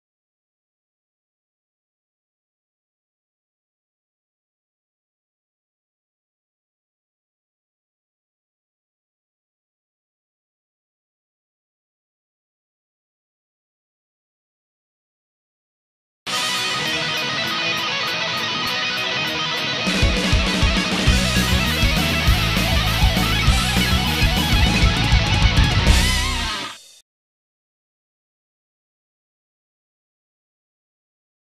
ＢＯＳＳ ＳＤ－１（スーパーオーバードライブ）を使った演奏
ボスのギターエフェクターＳＤ－１（スーパーオーバードライブ）を使った音作りの実験動画です。